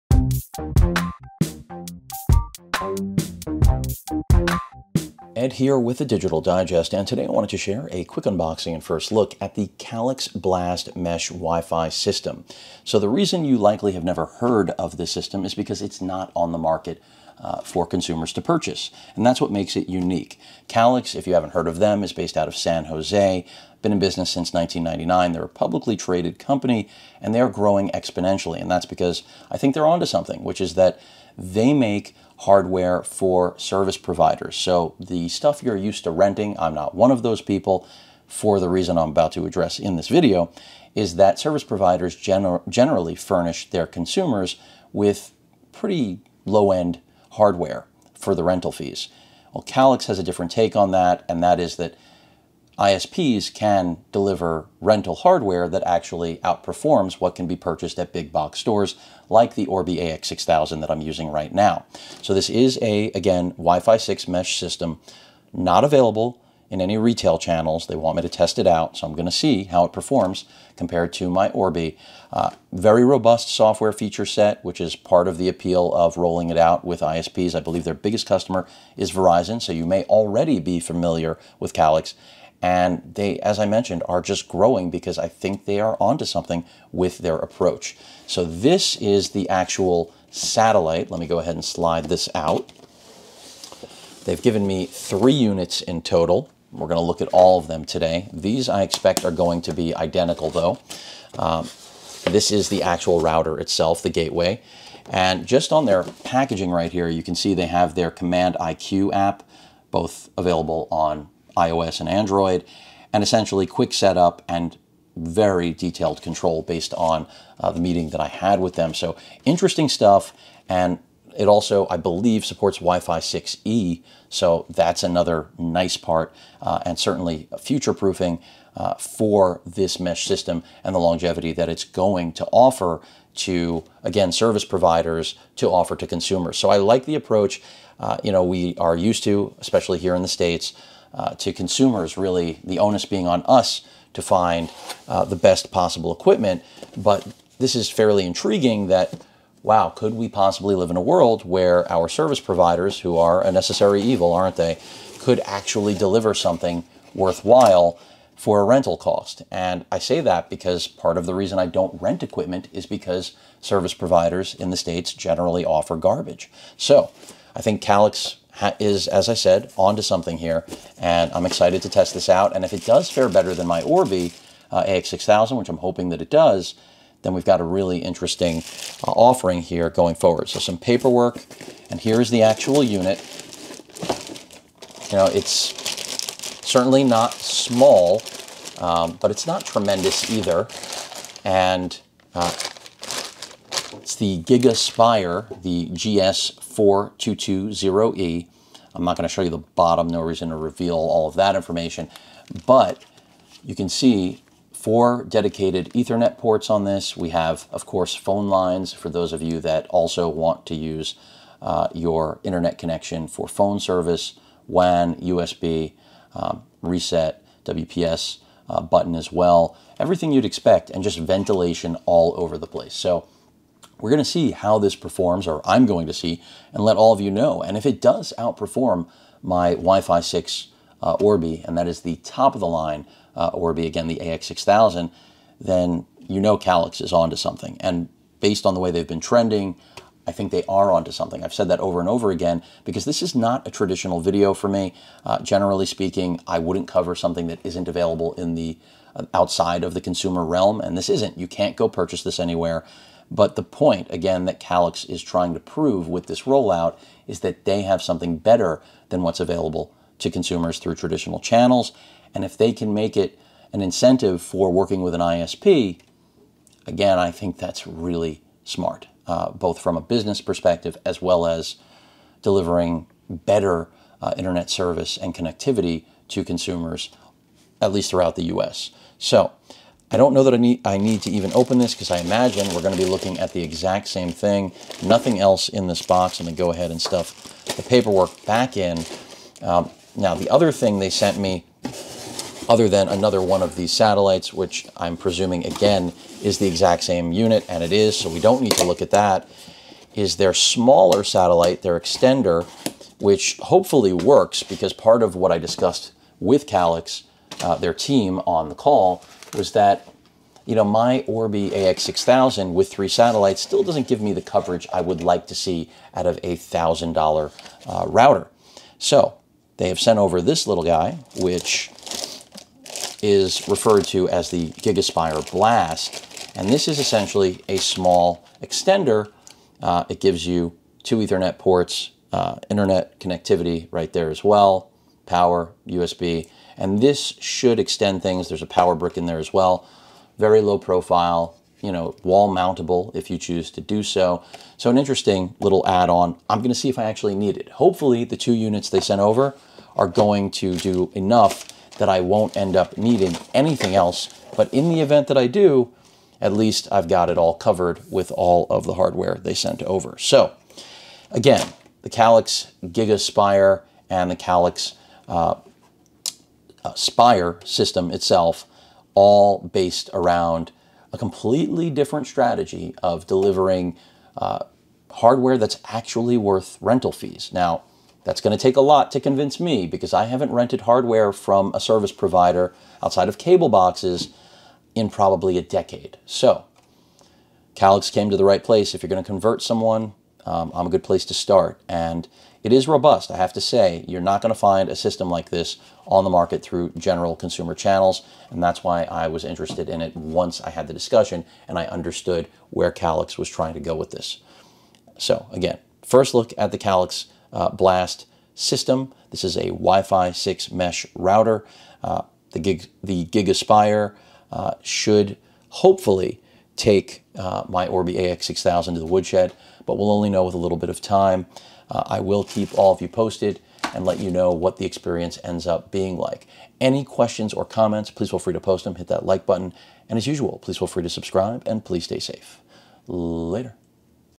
Calix GigaSpire Blast U6 and U4 WiFi 6 Mesh Router System Unboxing and First Look
Here is my Unboxing and first look at the Calix GigaSpire Blast U6 mesh wireless router system.